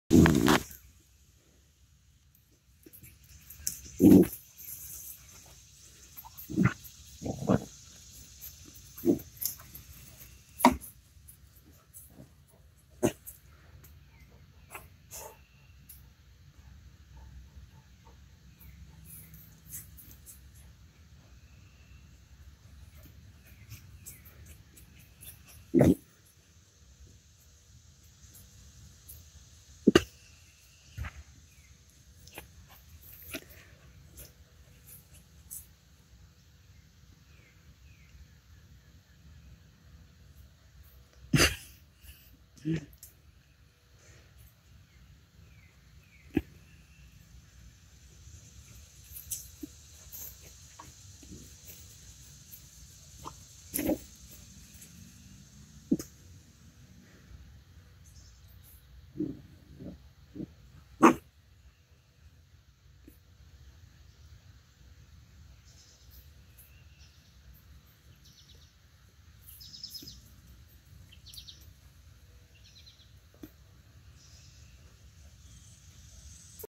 Newfie Ellie takes time out for tail taming
Ellie is almost 16 weeks old now and still is fascinated with the flutter of white that catches her attention when she bounces and plays.  Who cam blame her it's quite an attractive marking for a Landseer.
♥️ my Newfies ❣